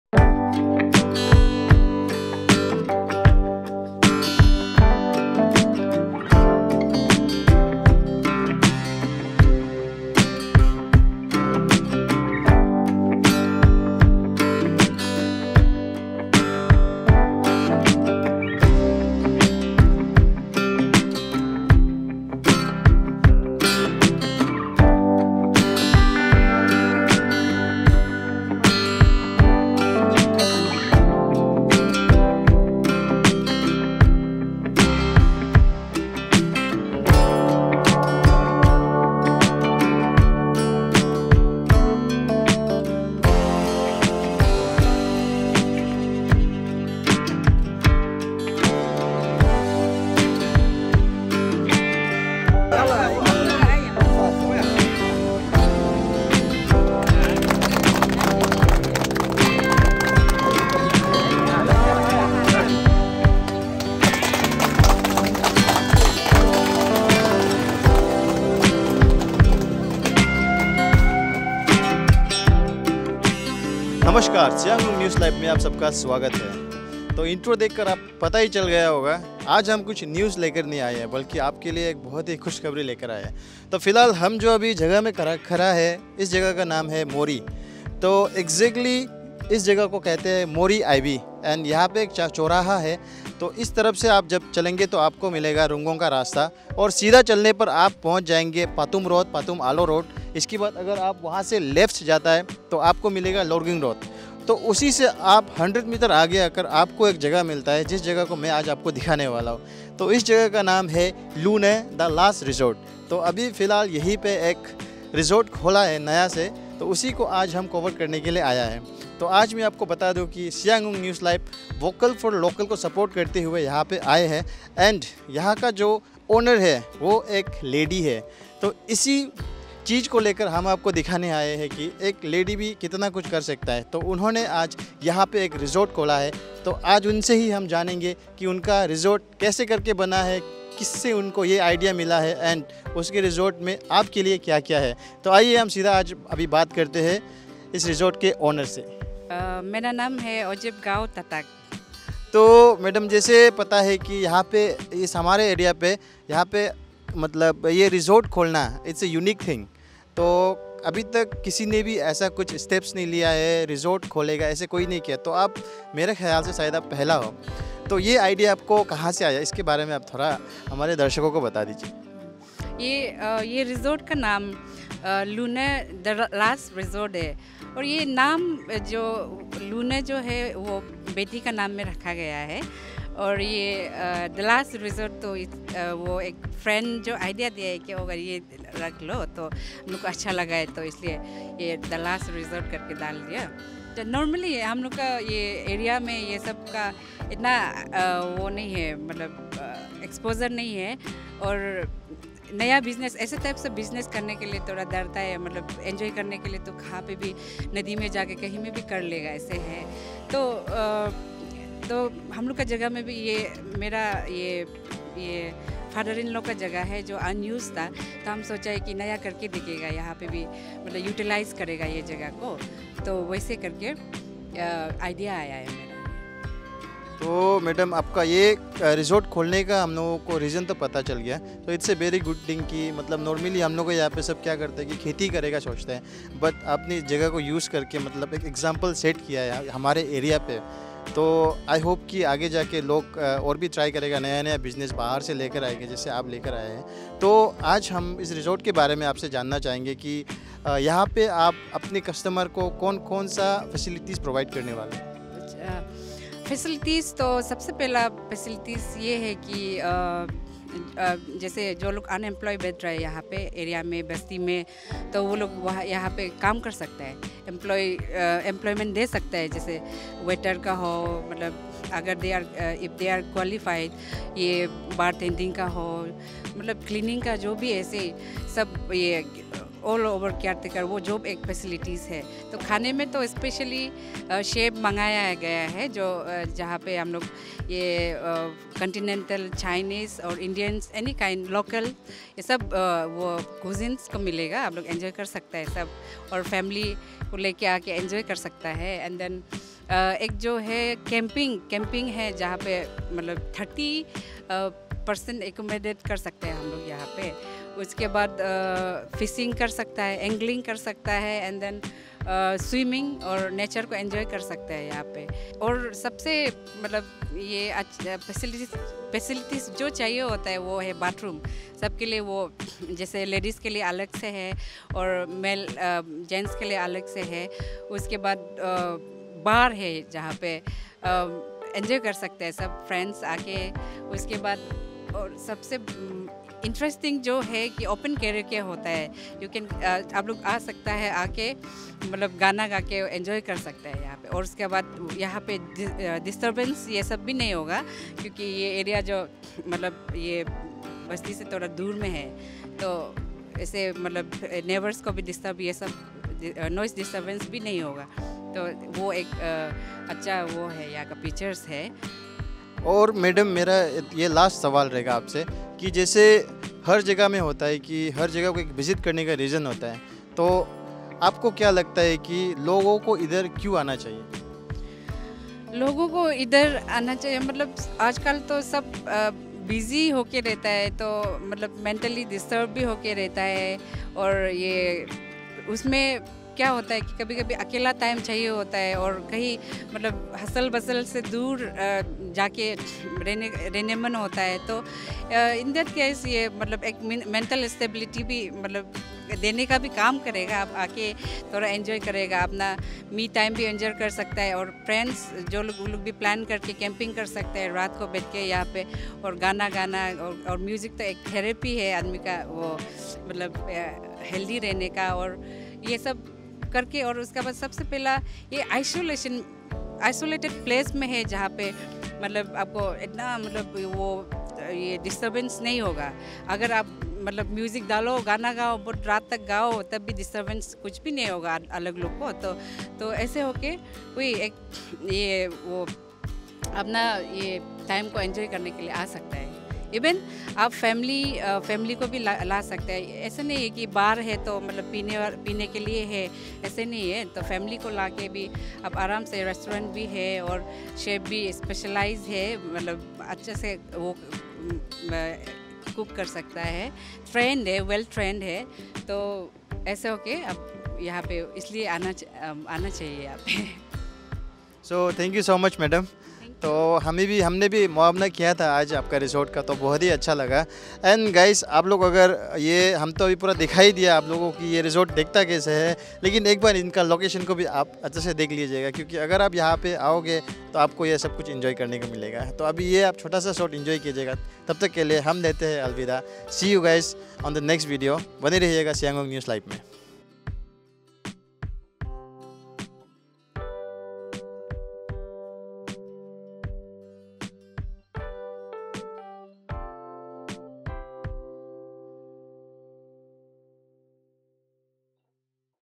NEW RESORT OPENING AT MORI VILLAGE TRI-JUNCTION || SIANG EUNG NEWS LIVE
LU'NE THE LAST RESORT IS NOW OPEN FOR PUBLIC AT MORI TRI-JUNCTION, WITH A VERY FRIENDLY AND FAMILY ENVIRONMENT AT THE BANK OF SIRIT RIVER.